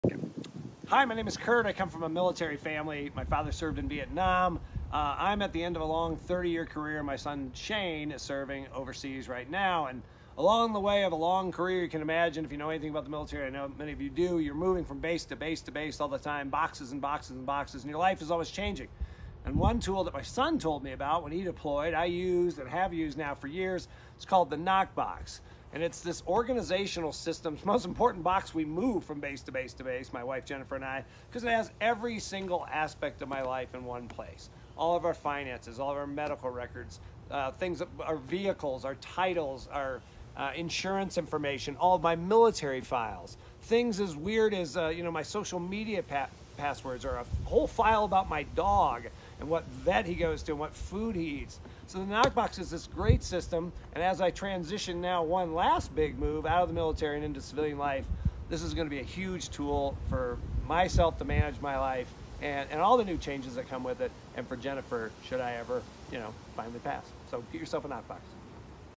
Organization Solution For Families
🇺🇸 Honoring Curt and our Military Heroes this 4th of July 🇺🇸

What is a Nokbox?

It’s an organizational system that helps your Next of Kin (NOK) avoid a scavenger hunt through your files, phone, home, possessions, computer, and paperwork once you have passed away.

Everyone in the family can benefit from having a Nokbox

20% Military Discount
or

Nokbox: Organization for Your Next of Kin